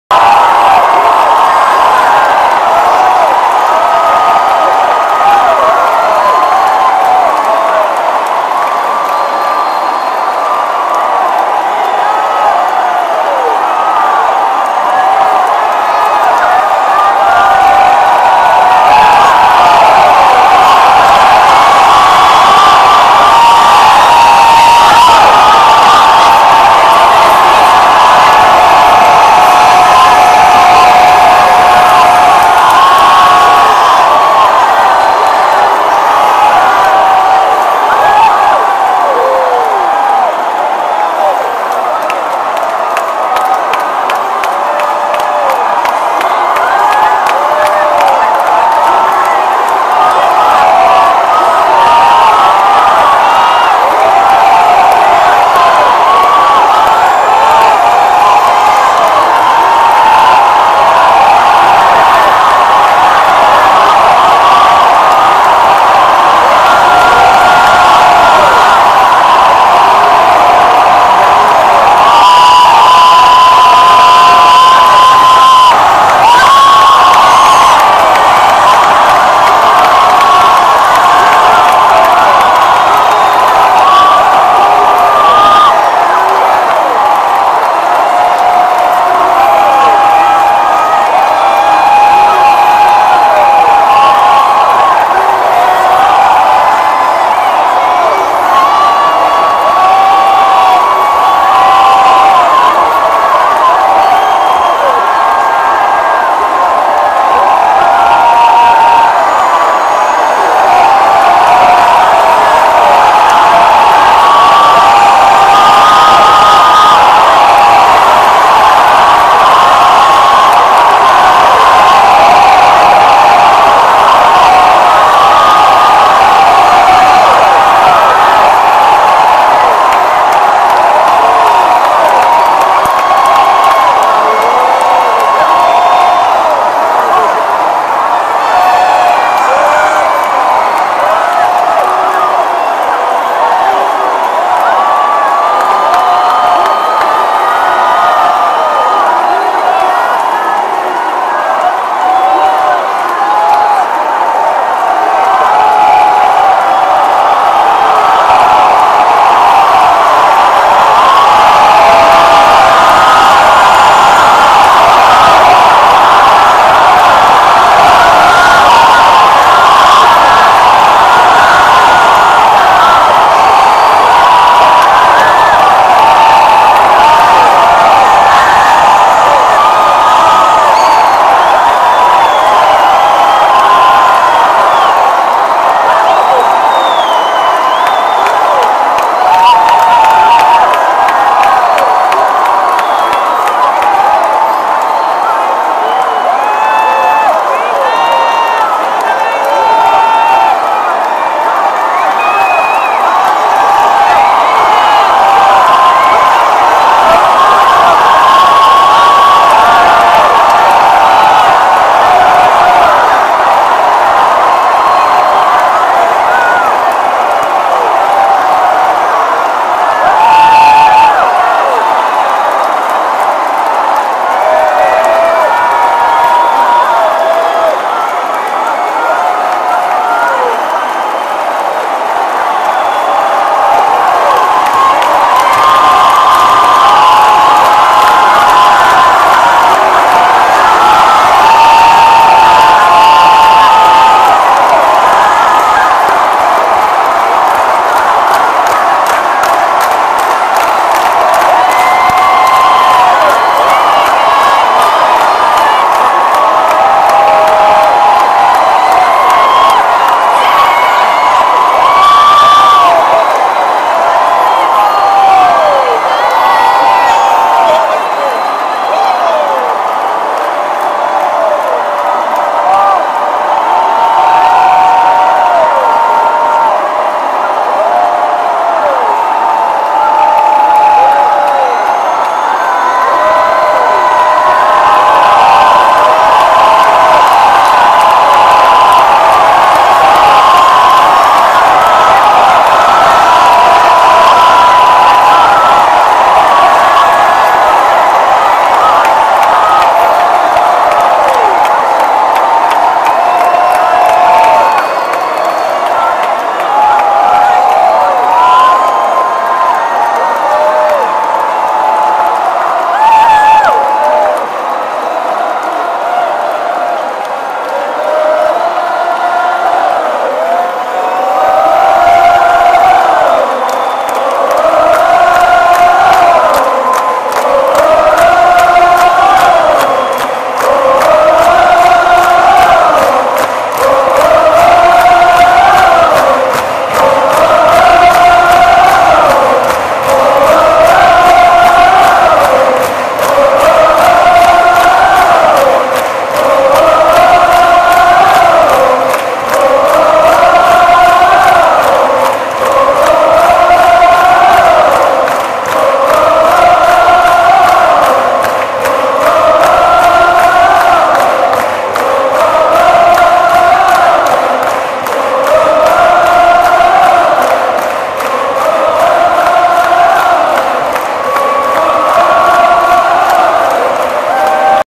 Roar of Praise at Jesus Culture Awakening 2011 - Chicago
This is what happens when the praise of the crowd overtakes the band.  This is only 6 minutes of what was about 15 minutes of the crowd's response to give Jesus praise at Jesus Culture's Awakening 2011 in Chicago!